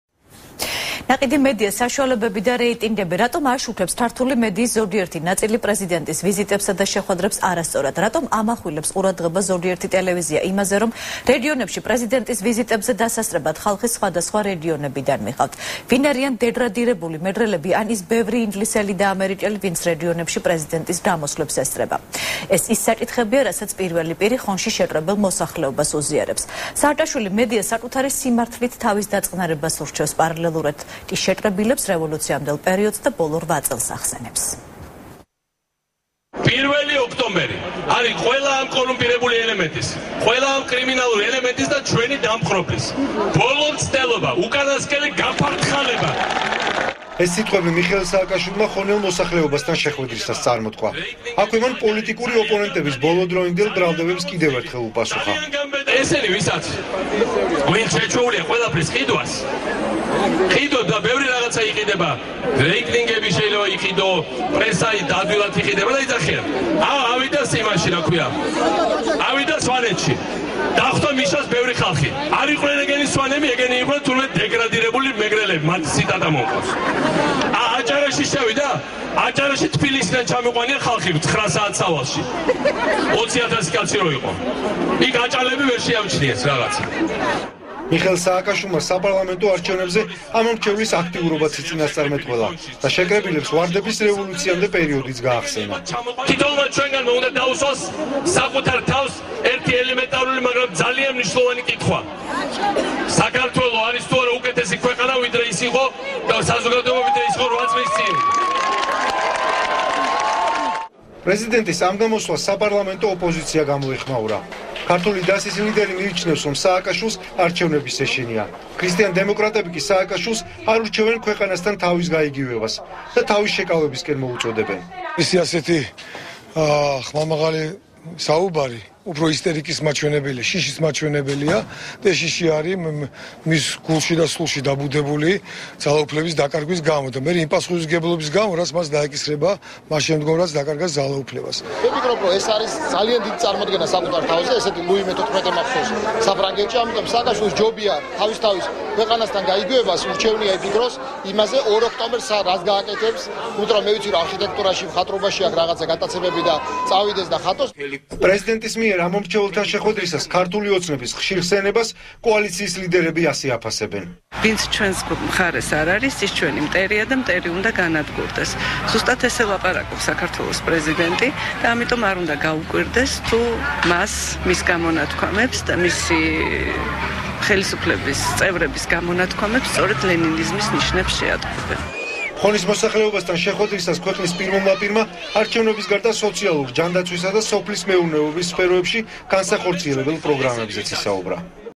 ახალი 9 | პრეზიდენტი ხონში | 25.08.12
ნაყიდი მედია საშუალებები და რეიტინგები. რატომ აშუქებს ქართული მედიის ზოგიერთი ნაწილი პრეზიდენტის ვიზიტებსა და შეხვედრებს არასწორად, რატომ ამახვილებს ყურადღებას ზოგიერთი ტელევიზია იმაზე, რომ რეგიონებში პრეზიდენტის ვიზიტებზე დასასწრებად ხალხი სხვადასხა რეგიონებიდან მიჰყავთ.  ვინ არიან დეგრადირებული მეგრელები  ან ის ბევრი ინგლისელი და ამერიკელი, ვინც რეგიონებში პრეზიდენტის  გამოსვლებს ესწრება - ეს ის საკითხებია, რასაც პირველი პირი ხონში შეკრებილ მოსახლეობას უზიარებს.  სააკაშვილი მედიას საკუთარი სიმართლით თავის დაწყნარებას ურჩევს, პარალელურად კიშეკრებილებს  რევოლუციამდელ  პერიოდსა და ბოლო რვა წელს ახსენებს.